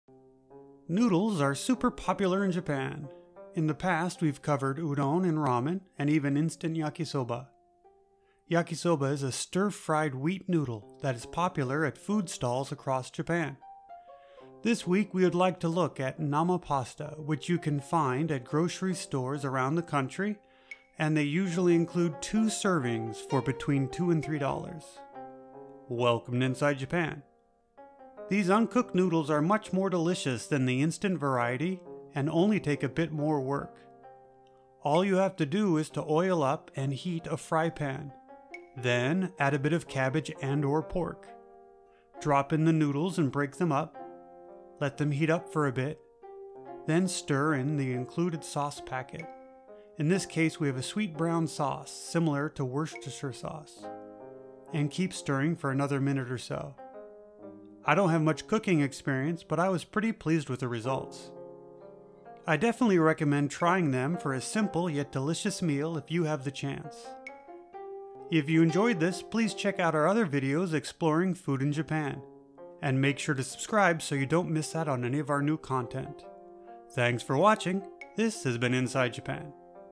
LETS MAKE YAKISOBA - TASTE OF JAPAN - EASY MEALS
Join us as we take a look at cooking the Yakisoba noodles found in grocery stores across Japan.

For more information about Inside Japan please visit our

Animation/Editing - Derek Edwards
Category
Travel & Events
License